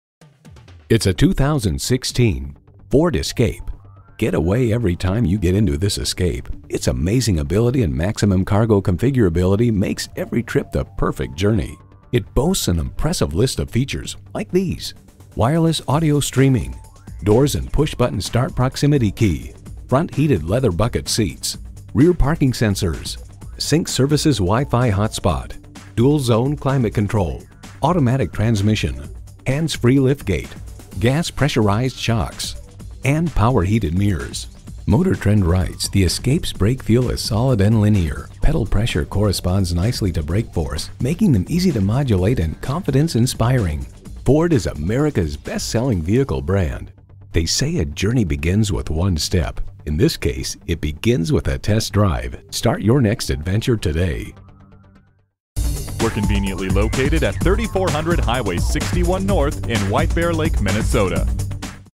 Used 2016 Ford Escape Saint Paul White-Bear-Lake, MN W92341M - SOLD
Year: 2016
Make: Ford
Model: Escape
Trim: 4WD 4dr Titanium 2.0l 4cyl
Engine: 122 L 4 Cyl. Cyl.
Transmission: 6-Speed A/T
Color: Ruby Red Metallic Tinted Clearcoat
Interior: Charcoal Black
Mileage: 57855
Stock #: W92341M
VIN: 1FMCU9J99GUC37524
Ecoboost Titanium with a lifetime powertrain warranty that covers the engine, the turbo, the all wheel drive, the transmission and more. Lots of extra features not standard on lessor models like heated leather and much more. CARFAX says it's a one owner too. Money back guarantee!

Address:
3400 Highway 61 North